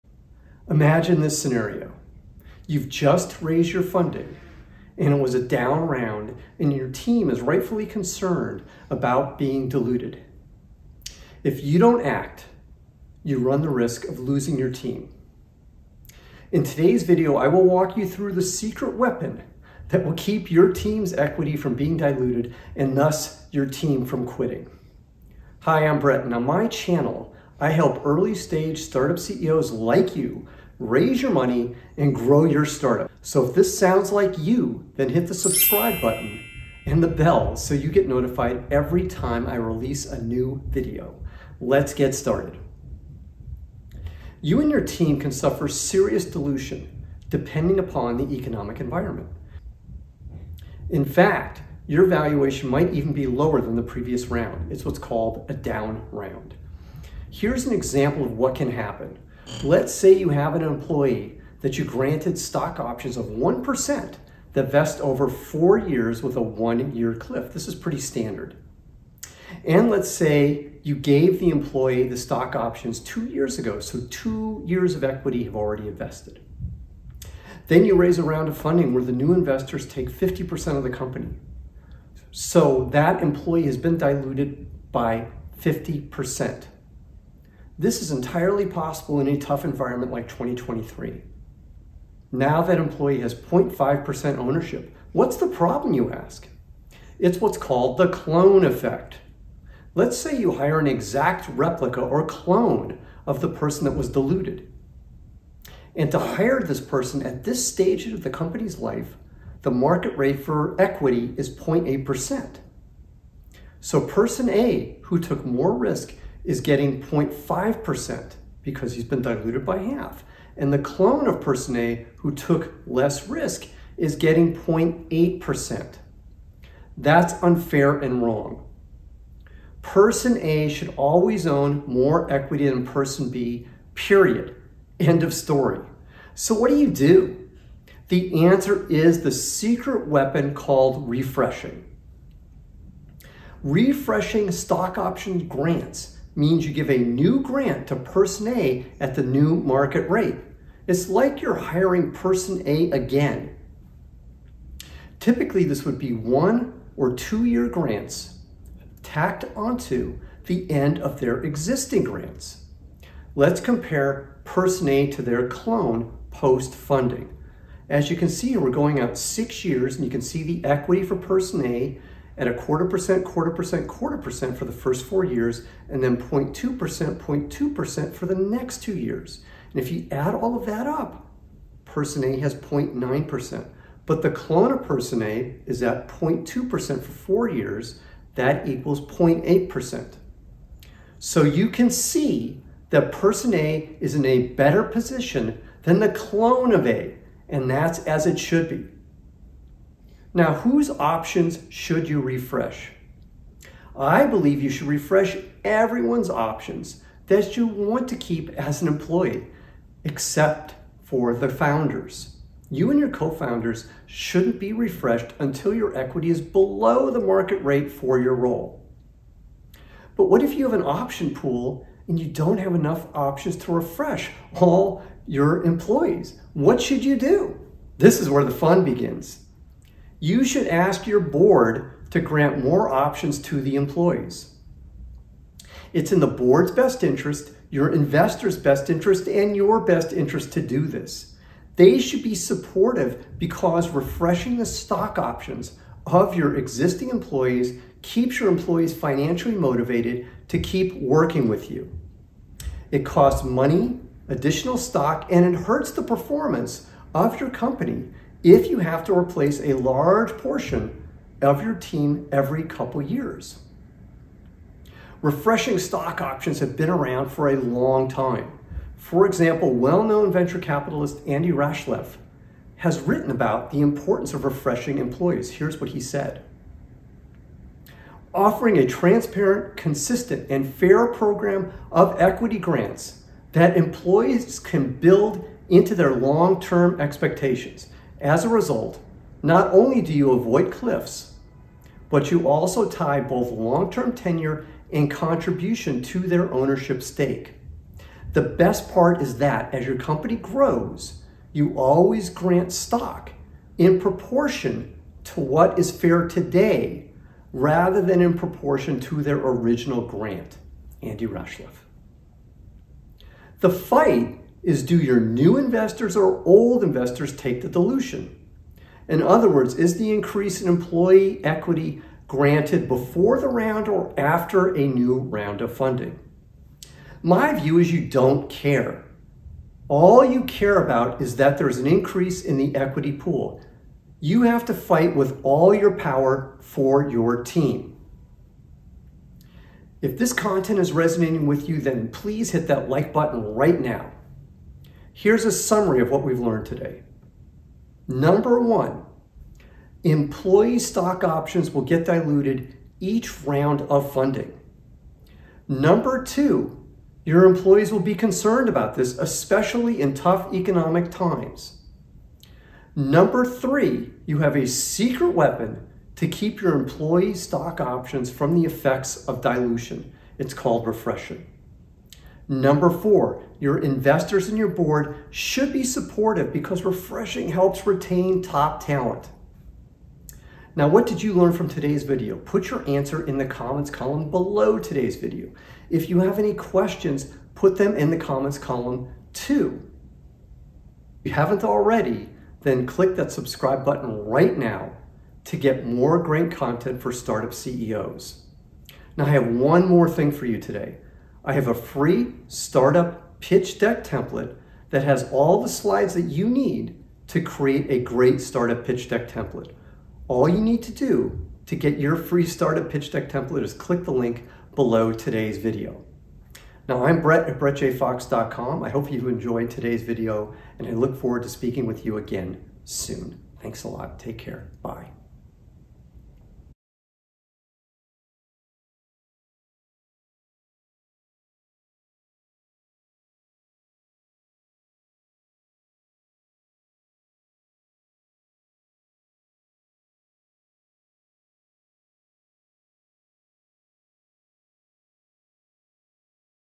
Protecting Startup Employee Stock Options From Dilution, Explained
Your employees are going to be really worried about their stock options being diluted after a round of funding.  This is especially true in bad economic times when you might take a down round with heavy dilution.

However, there is a secret weapon you can employ to reduce the dilution you and your employees will take. In today’s video, I’ll share with you the exact strategy you can employ.  I hope you like it.